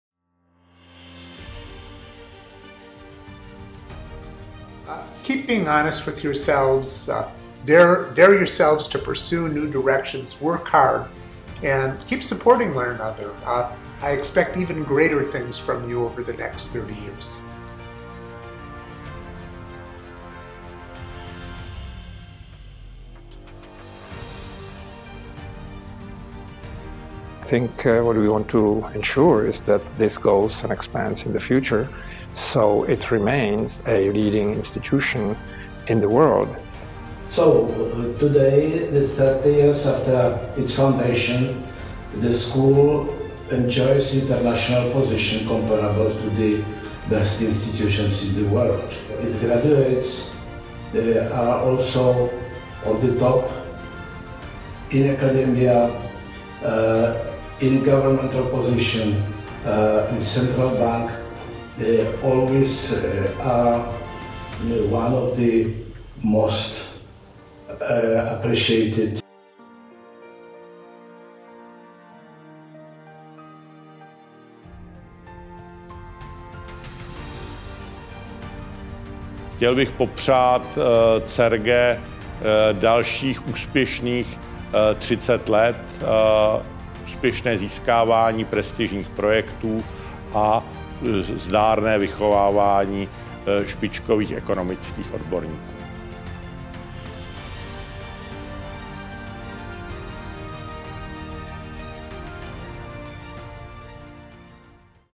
30th Anniversary - Wishes
CERGE-EI celebrates its 30th anniversary this year. Our achievements rely on the support of our parent institutions and the generous support of our dedicated donors and members of wider CERGE-EI community. Thank you!